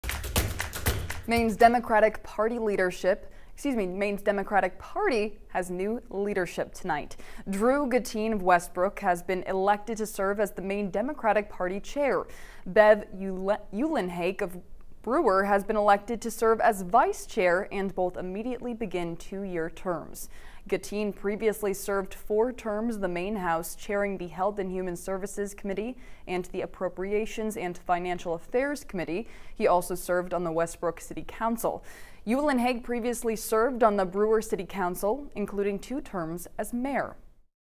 New Maine Dems party chair
Maine's Democratic Party has new leadership Sunday night.